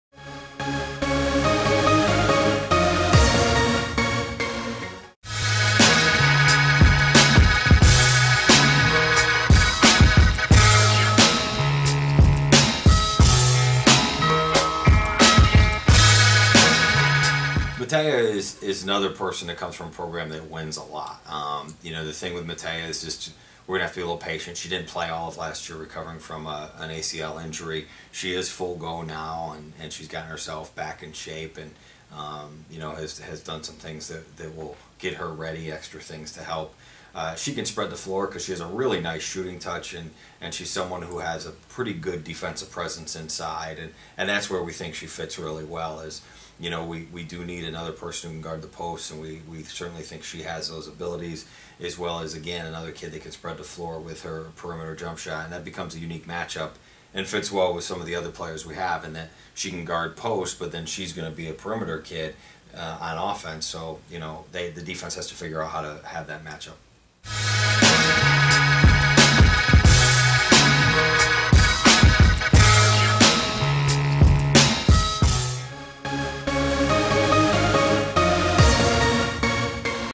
Bonnies Spotlight: Matea Britvar
For the next 10 days, the Bonnies Spotlight will feature one of the new players on the St. Bonaventure men's or women's basketball teams. Video #2 features women's Freshman Matea Britvar